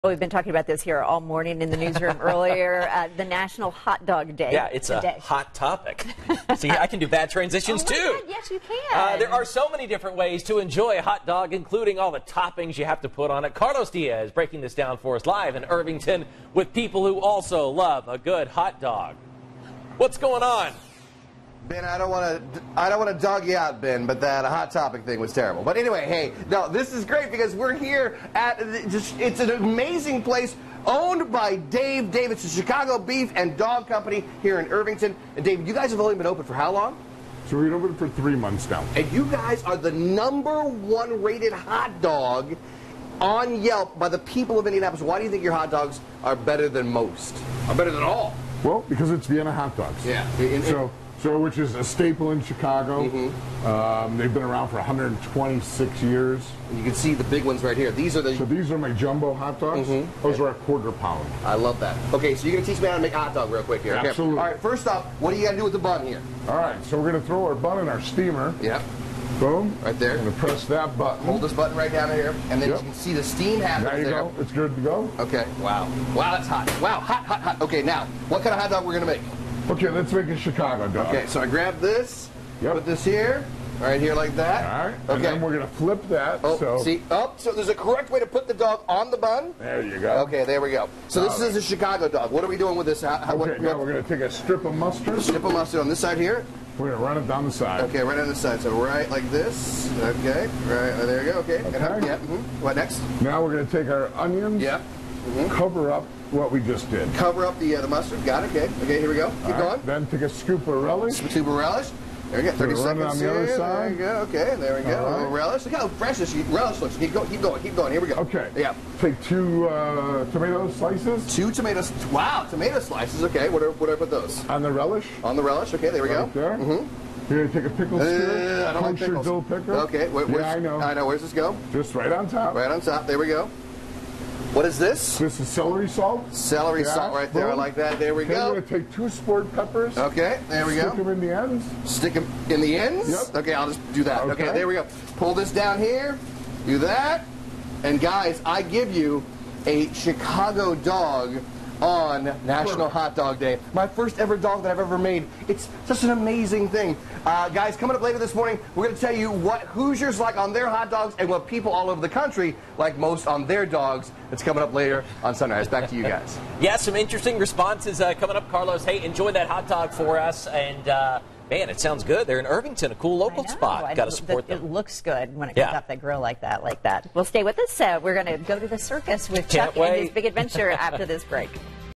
National Hot Dog Day
There are so many different ways to enjoy a hot dog, including all the toppings you can put on it! Sunrise's Carlos Diaz reports.